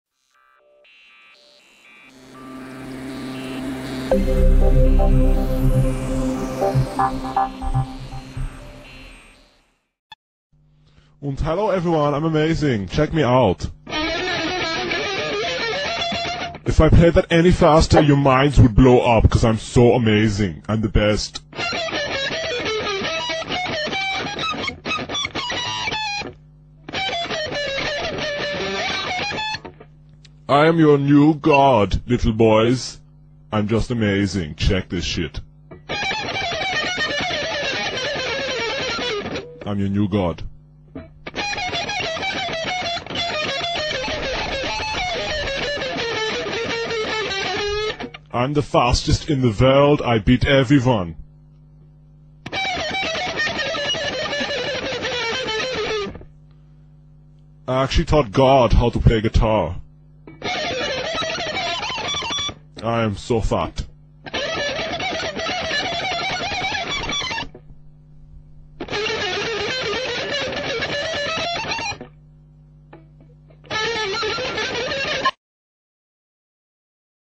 Yngwie Parody
This is a video that will force your body to reach 1000 DJ (Hearing 1000 dad jokes all at one) and will make you laugh. The person In this video is Yngwie Malmsteen a Swedish guitarist.